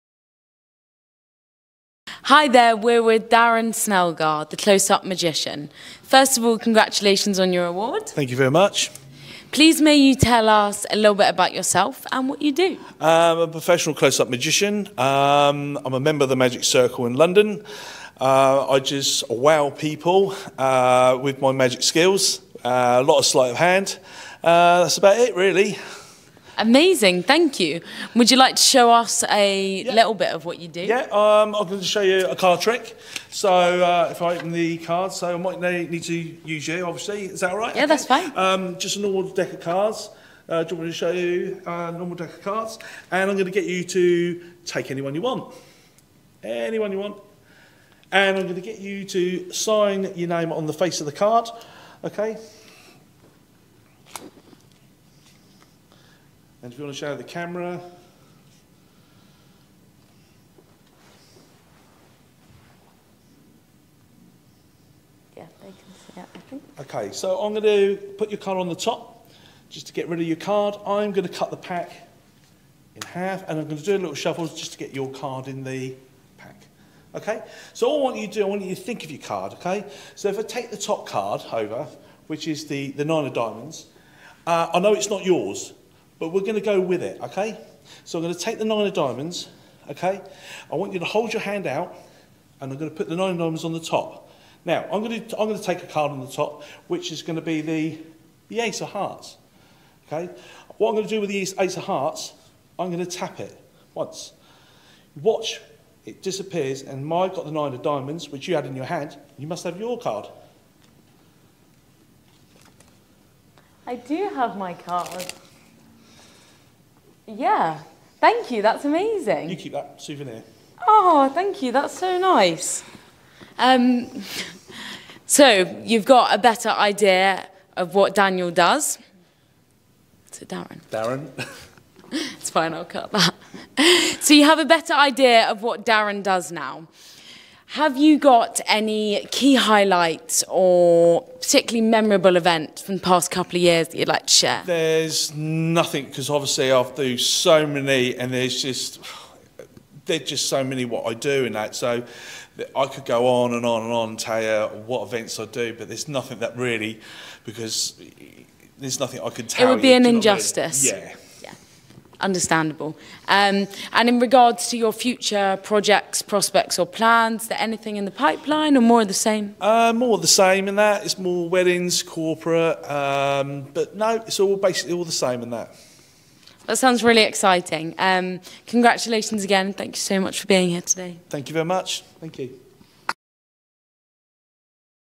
Winner in the South England Prestige Awards 2023/24
We invited winners in our South England Prestige Awards 2023/24 to Solent Hotel and Spa on the 28th and 29th March 2024 to meet the judges and celebrate their Win.